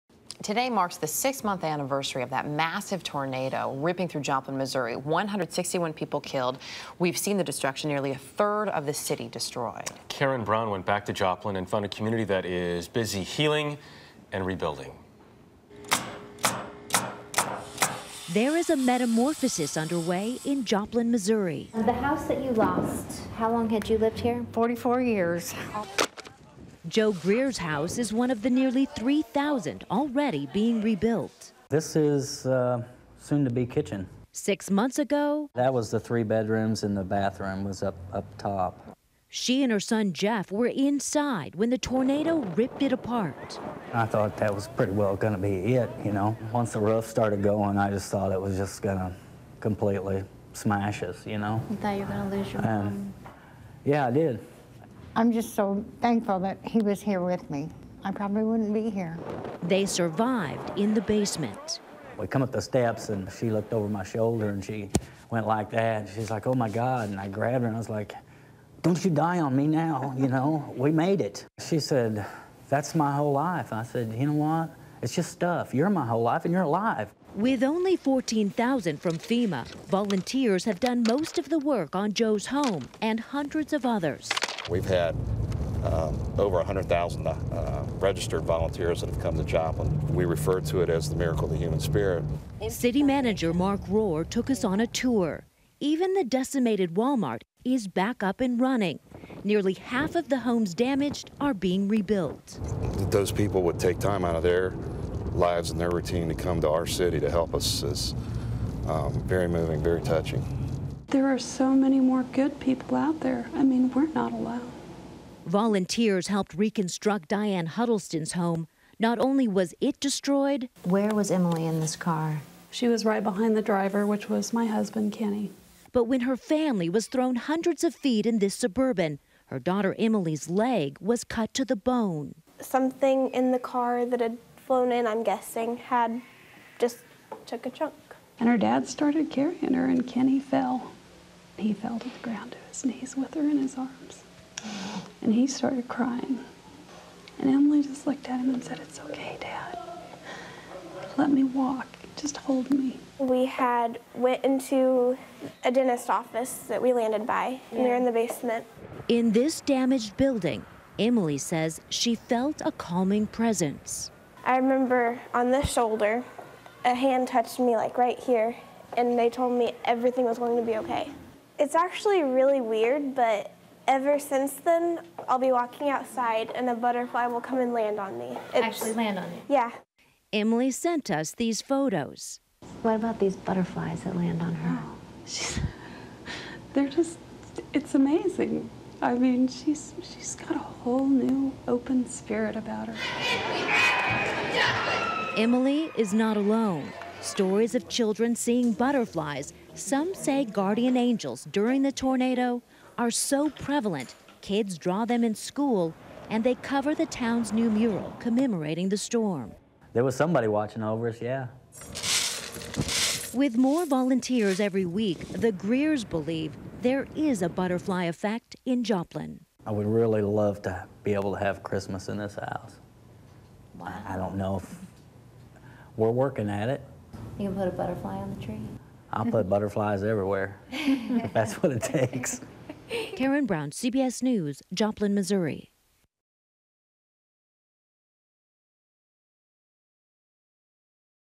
Joplin, Mo. - Six months later
CBS News correspondent Karen Brown reports from Joplin, Mo. for a look at how the town is getting help to heal and rebuild six months after a tornado destroyed nearly one-third of the city and killed 161 people.